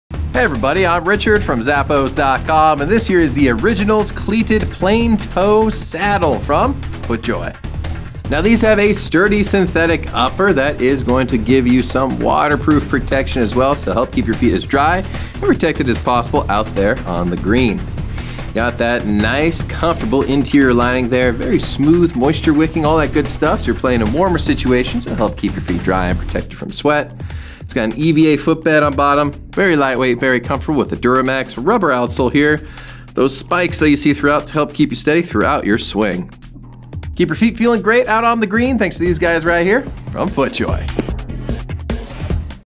FootJoy Originals Cleated Traditional Plain Toe Saddle SKU: 9086323
Step your game up with the FootJoy&reg; Originals Cleated Plain Toe Saddle golf shoe.
Synthetic upper with offers 1 year waterproof comfort, breathability, and durability.
Lace-up closure for a custom fit.
EVA Fit-Bed provides lightweight cushioning around the foot.
This DuraMax rubber outsole is a proprietary compound that provides turf gripping performance and durability.
Imported.

Product measurements were taken using size 11, width W. Please note that measurements may vary by size.